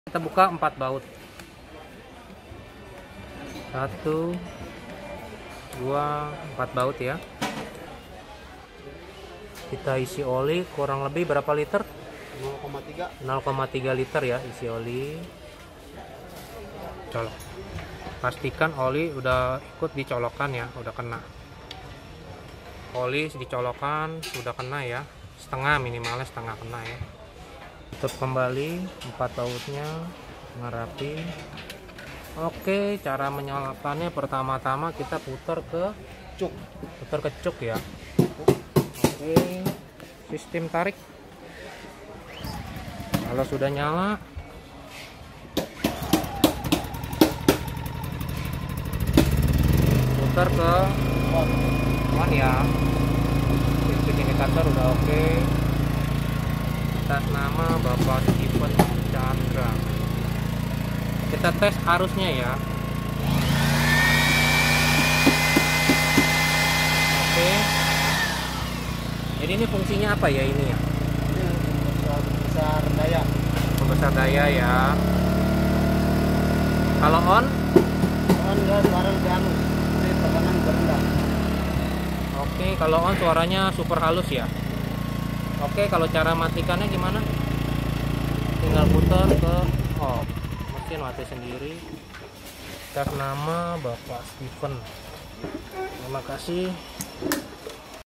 Super silent genset inverter 2000 watt 2.5kva Tsuzumi TIG 2000 IS
Gasoline silent inverter generator
merk: tsuzumi
type: TIG 2000 IS
rated power: 2000 watt